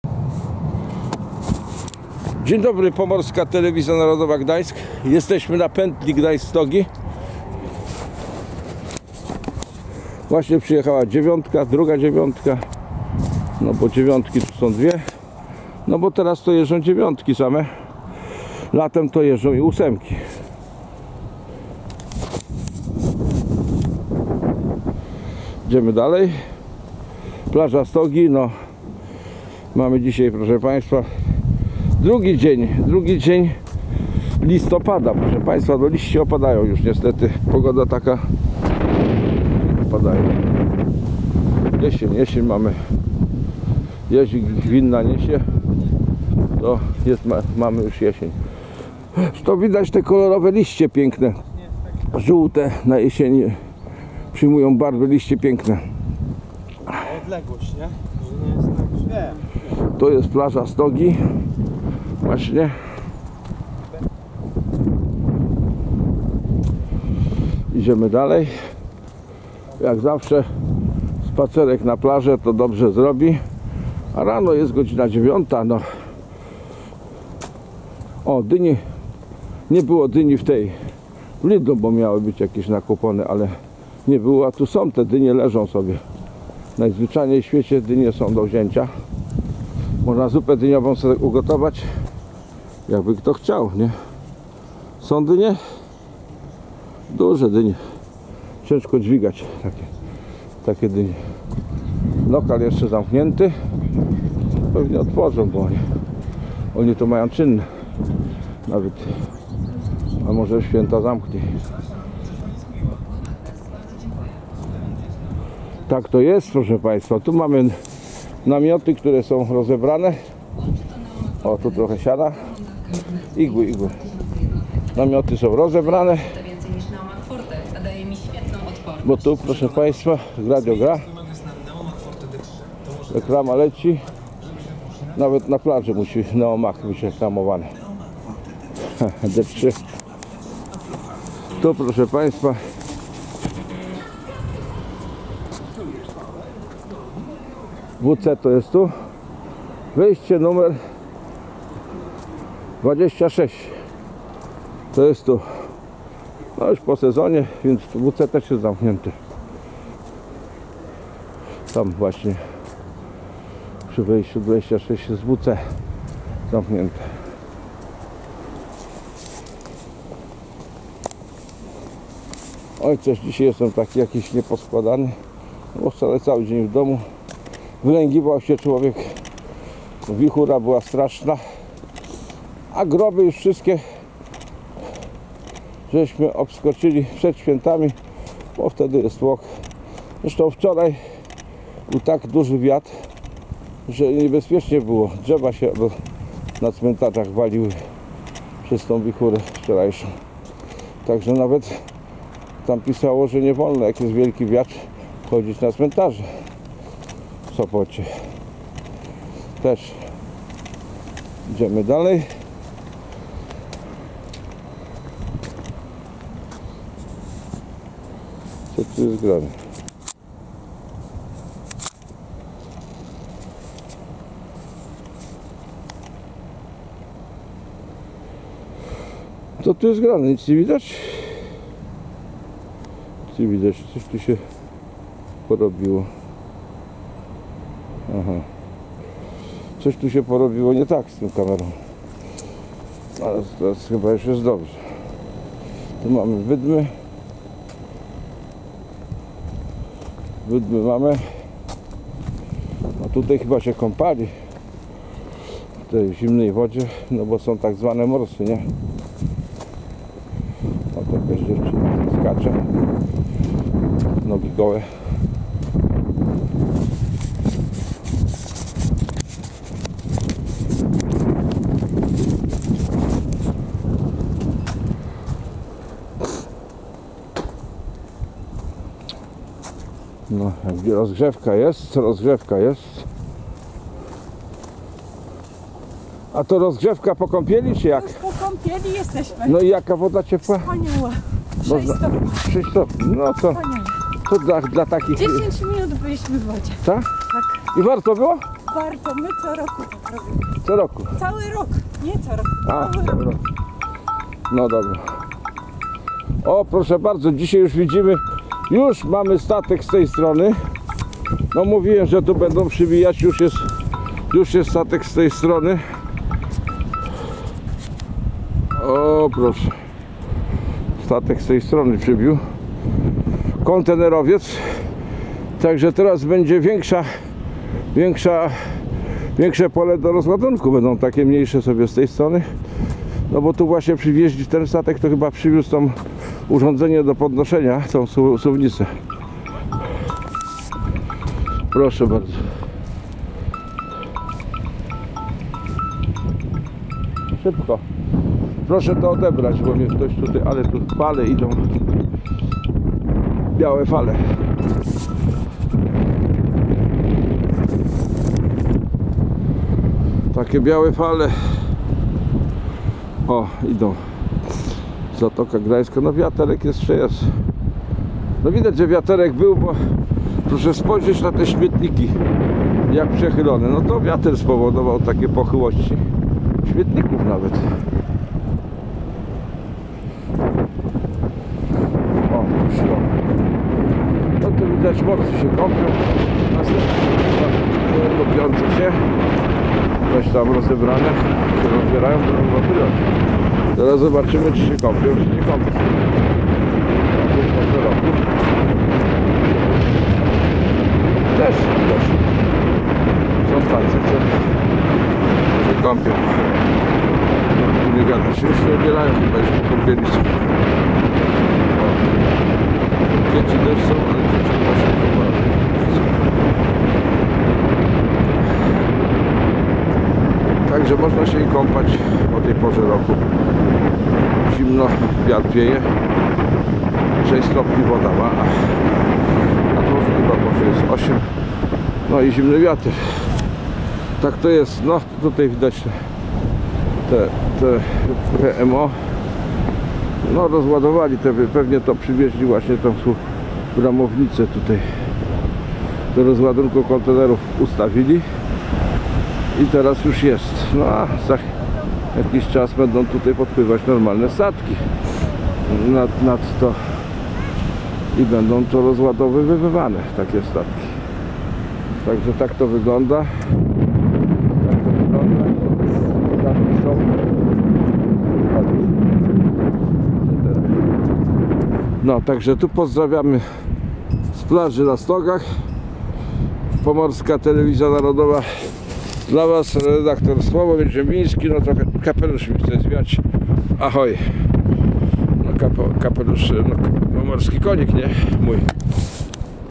PLAŻA STOGI 2 LISTOPAD, JEST JUŻ STATEK PRZY NOWYM NABRZEŻU KONTENEROWYM I NOWA SUWNICA PRZYPŁYNĘŁA?
PLAŻA STOGI jest dalej eksploatowane , bo jak widać kompielących sie tam nie .brakuje!?
Są  tacy ludzie co się kompią w Zatoce Gdanskiej cały rok i mówią ,  ze to służy to ich zdrowiu!?
Ponadto jak widac z Plaży przypłynął statek i zacumował przy niedawno pobudowanym nabrzeżu.
Teraz sa tam nowe suwnice  i będzie mogłó być jednos=cześnie rozładowywanych kilka statków z kontenerami w Porcie DCT!?